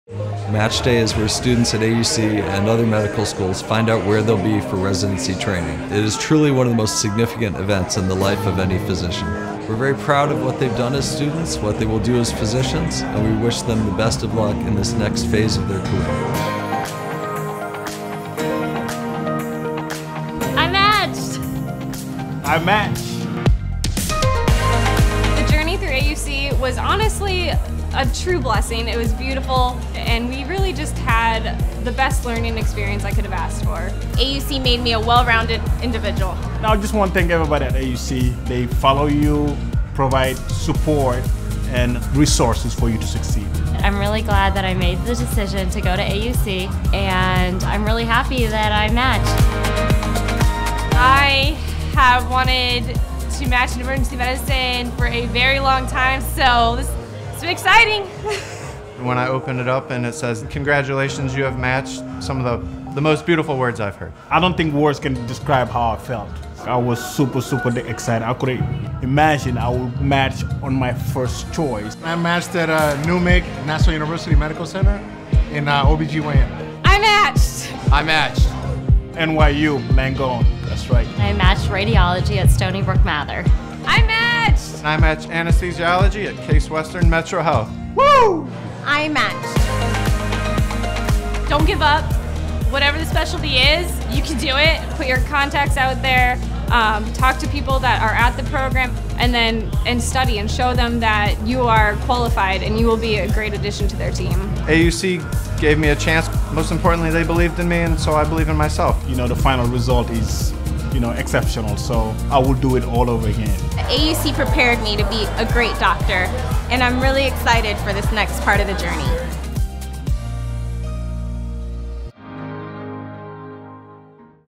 AUC Match Day 2018 - Miami
Match Day is one of the most exciting days in the journey to becoming an MD. This is where students find out where they matched and where they will continue on to practice in their chosen specialties. Miami 2018 recap is here for your pleasure! Watch as students share their emotions and where they will be heading next to follow their dreams.